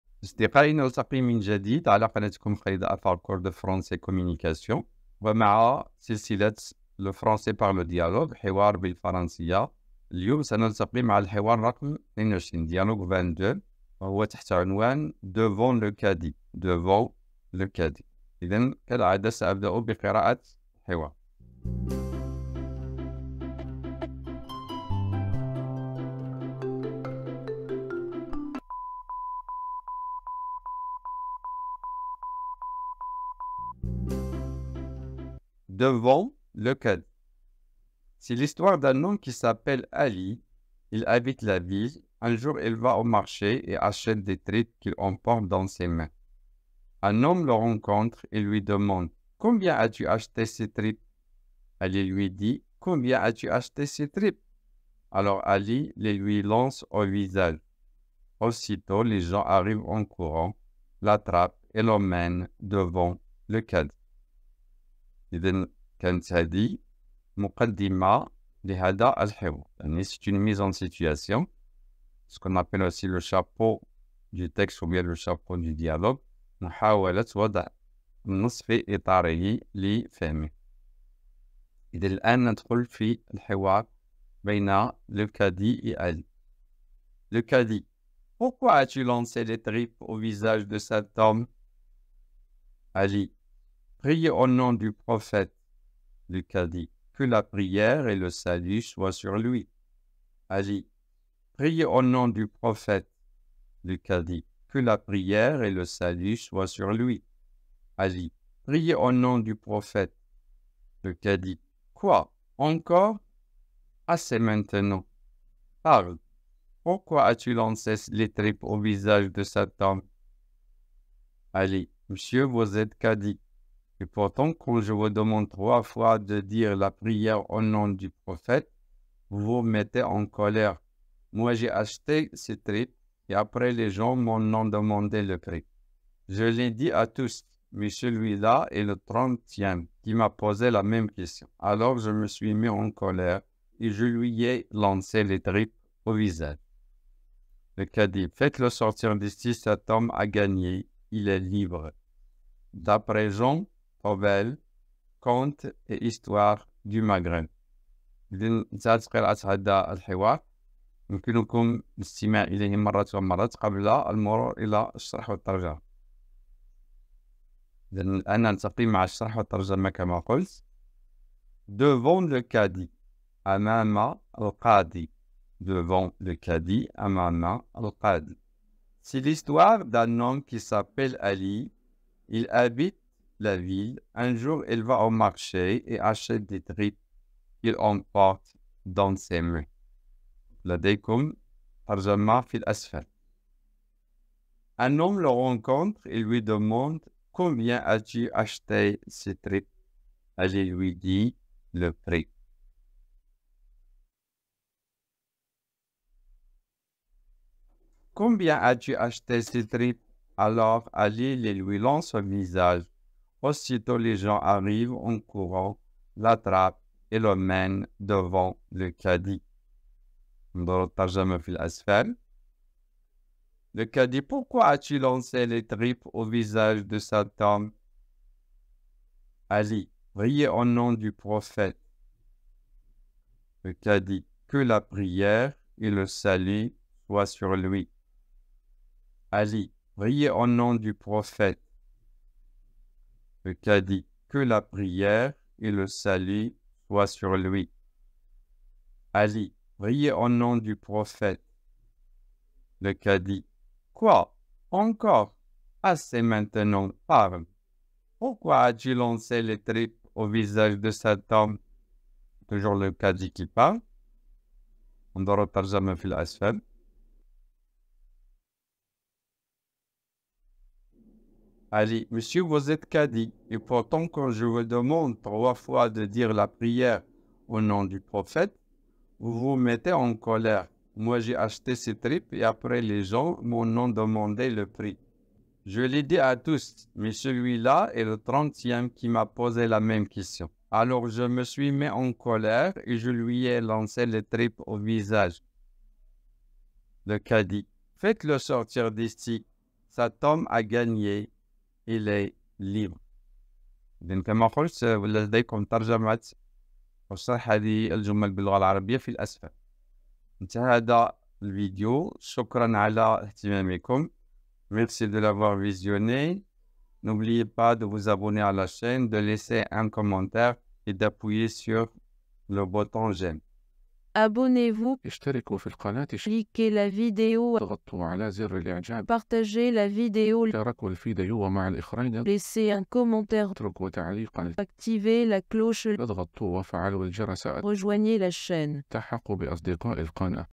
Dialogue 22 - Devant le juge💼 Communication en français au tribunal - Dialogues et situations
Cette vidéo vous guide à travers des dialogues et des situations spécifiques pour communiquer en français dans un contexte judiciaire. Vous apprendrez les expressions et le vocabulaire essentiels pour interagir avec le juge et participer aux procédures judiciaires.